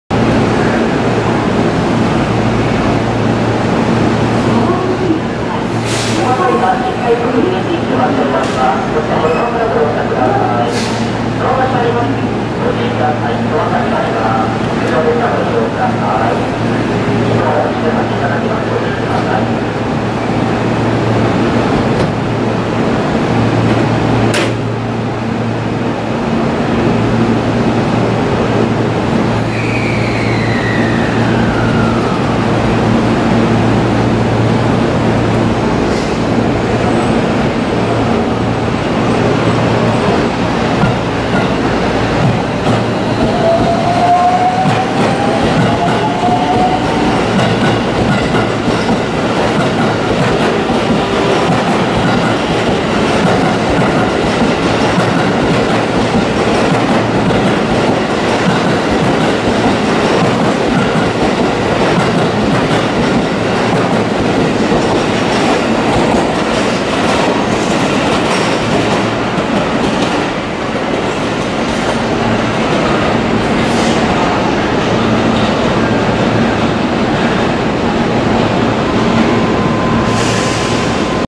JR京都線3455M新快速姫路行 京都駅発車
車両 223系2000番台近ホシW32編成+V16編成